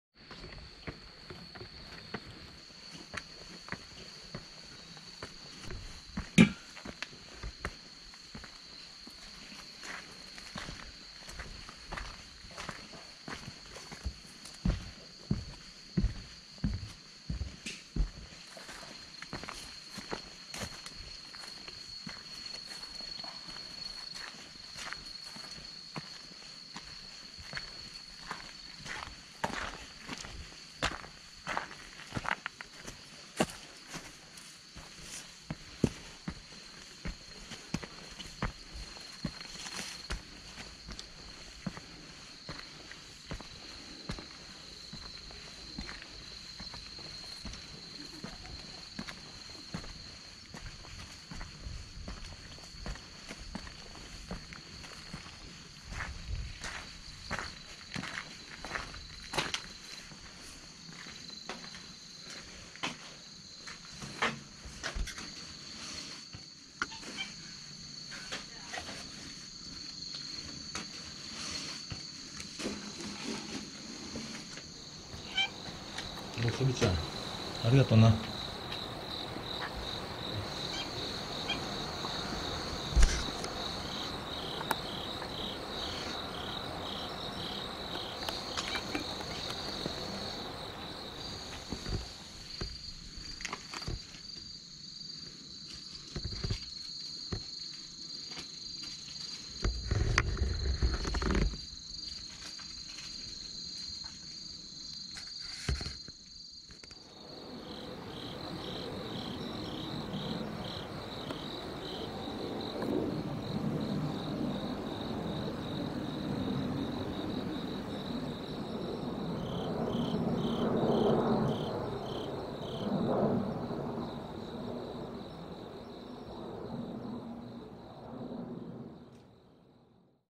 誰もいない暗い山道を歩いてた時に突然暗闇の中からあの猫が出て来て！🧐
いつもサプライズで当方を驚かすことが得意な忍者猫に！👓

【サブチャンネル❷】
シャムエとペポ猫地下室